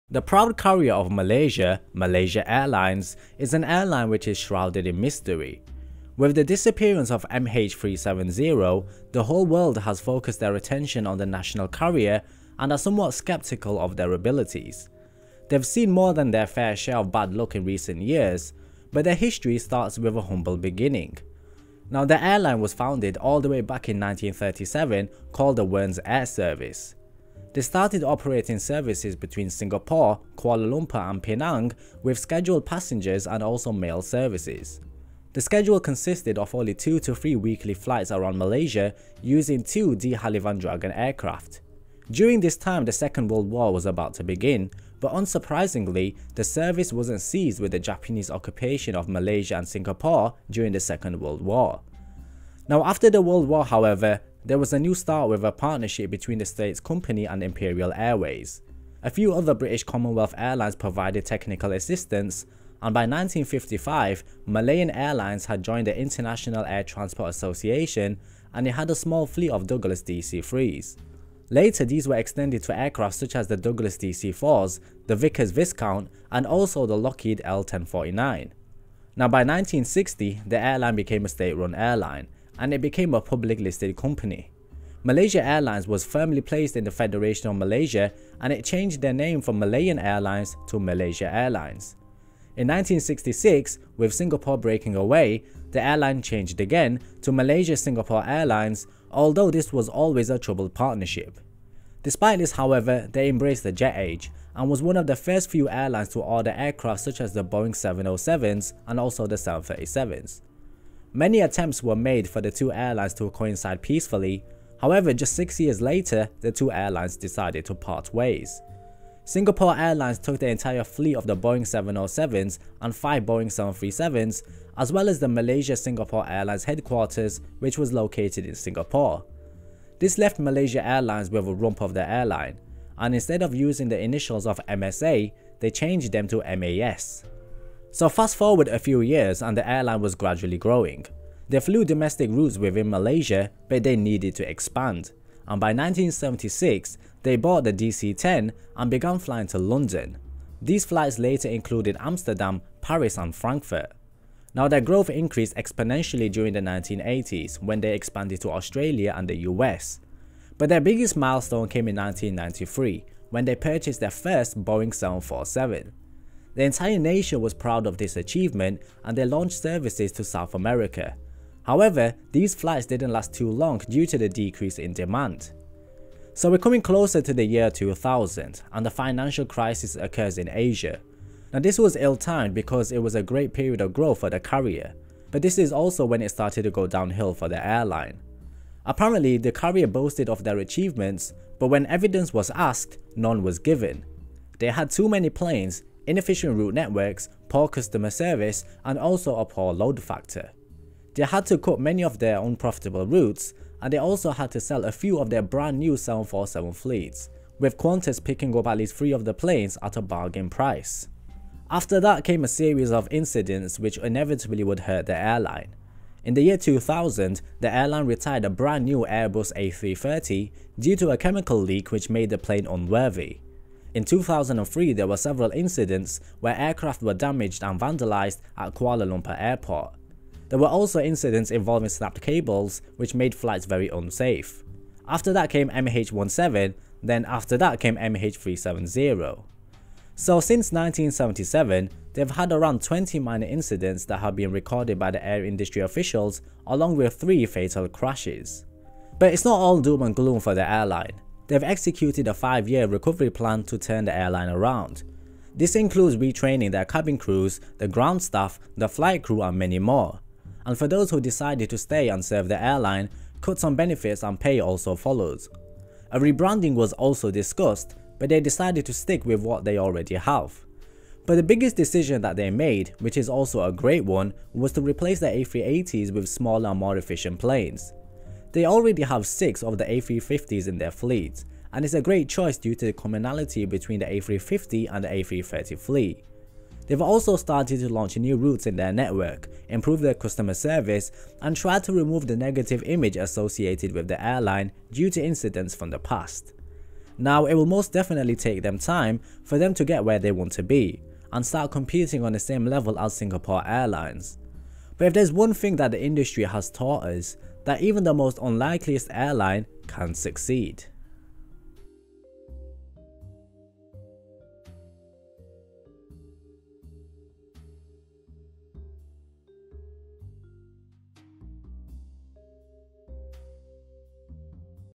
How Malaysia Airlines shaped Asian aviation
Malaysia Airlines, is an airline operating flights from Kuala Lumpur International Airport and from secondary hubs in Kota Kinabalu and Kuching to destinations throughout Asia, Oceania and Europe. Malaysia Airlines is the flag carrier of Malaysia and a member of the oneworld airline alliance. In this video we’ll discuss the airlines past and some of the difficulties that they faced.